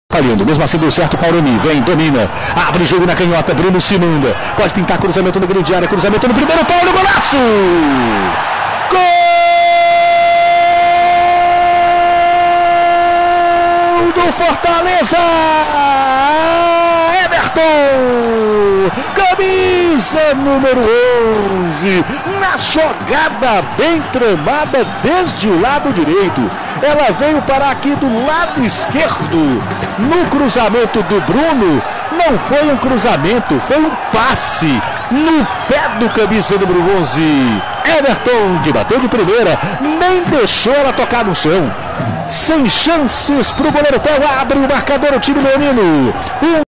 Campeonato Cearense - Fortaleza abre o placar da partida contra o Maranguape no PV - 15/03/2015
A partida é válida pela 5ª rodada da segunda fase do Campeonato Cearense 2015